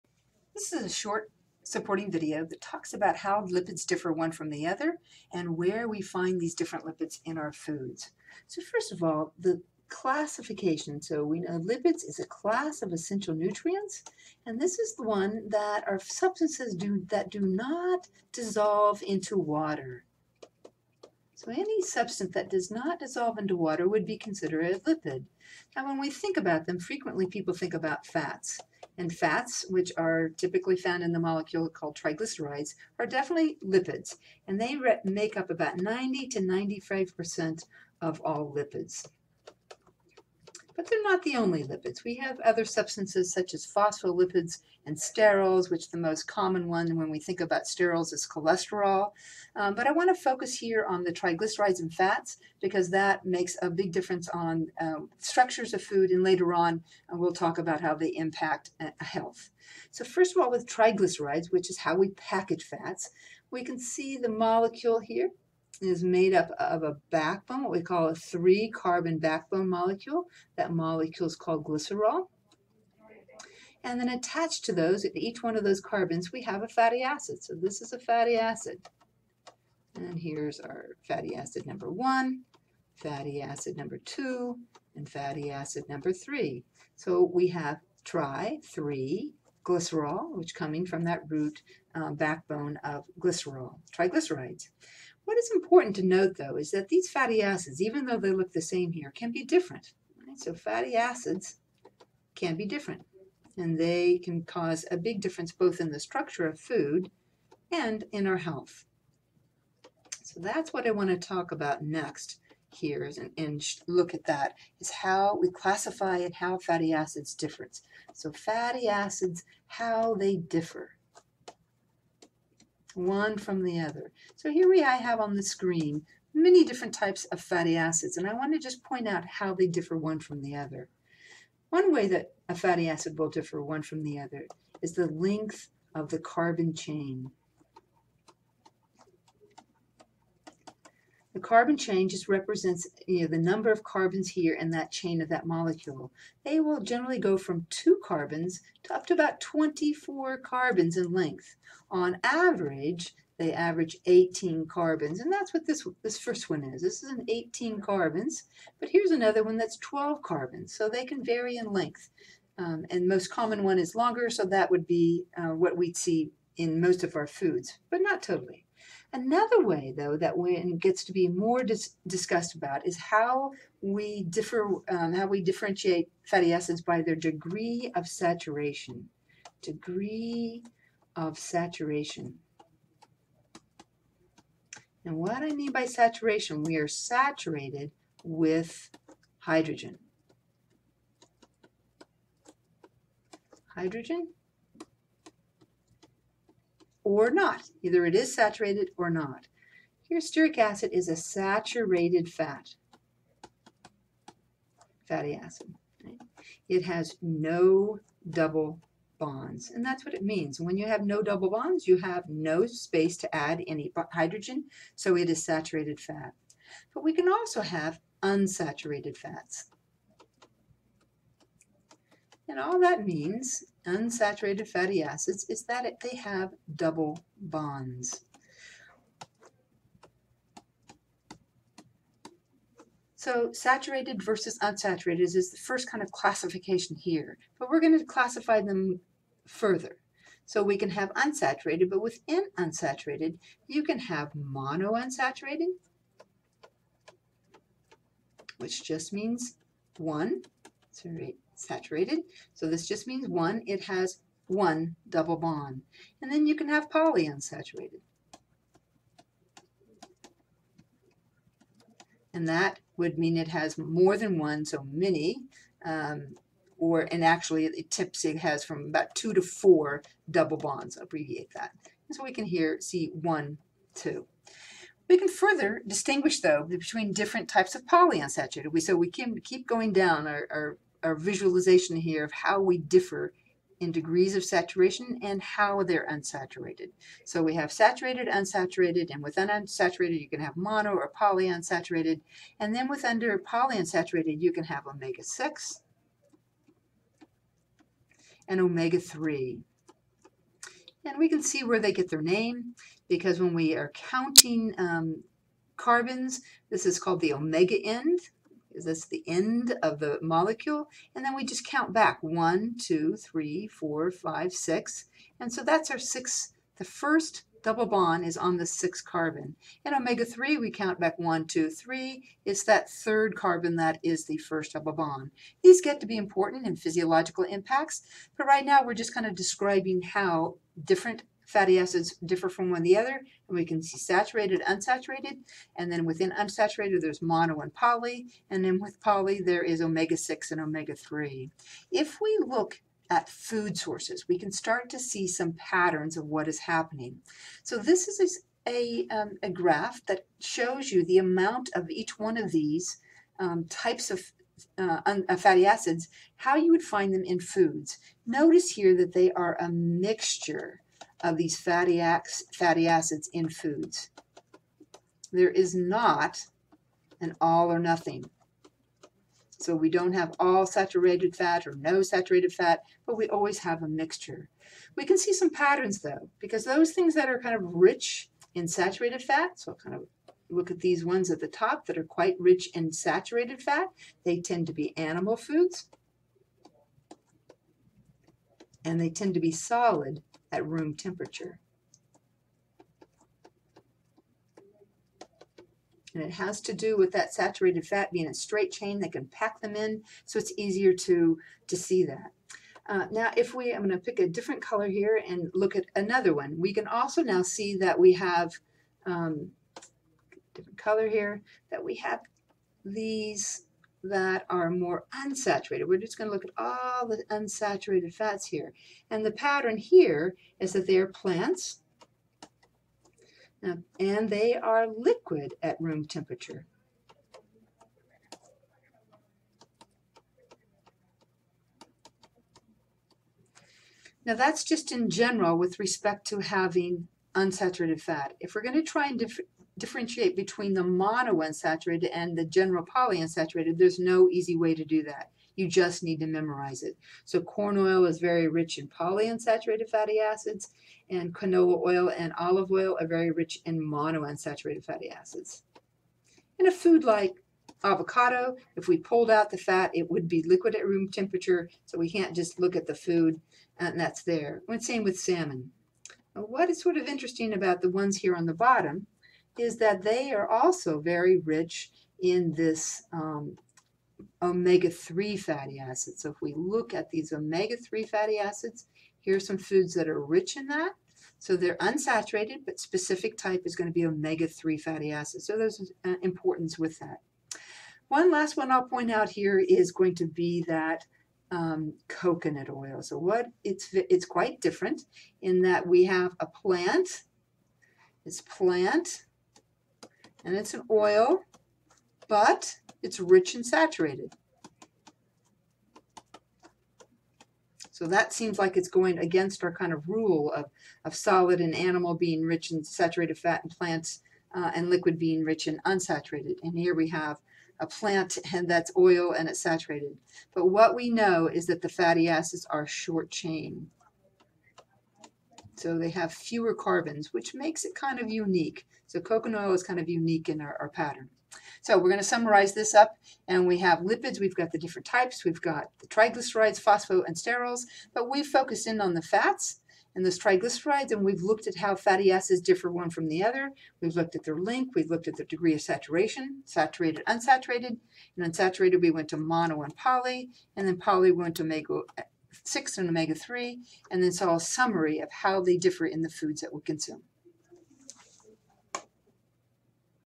Supporting video: Lipids
Supporting video discussing how lipids differ one from another.